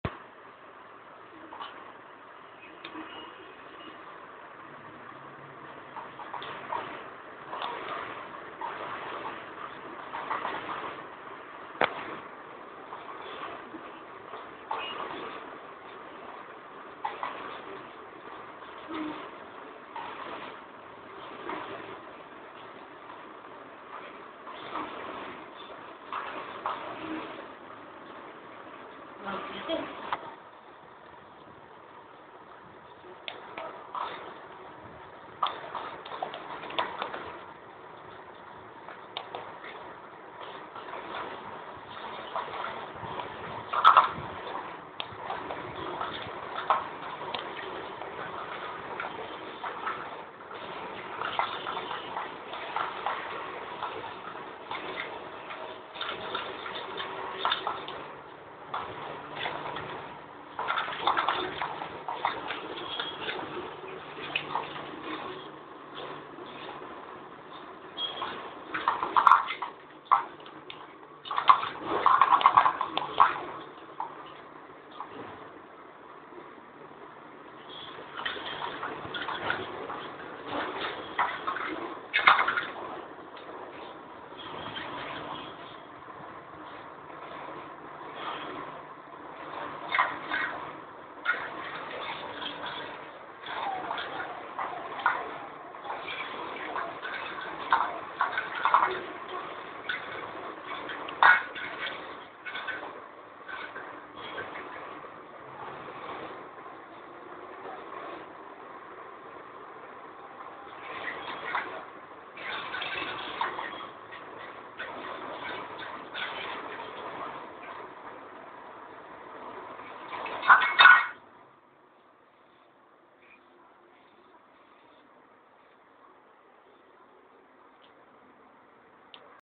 Dino the Labrador - Dining his food
Dino  is  voracious  eater  and loves eating both oriental and occidental food.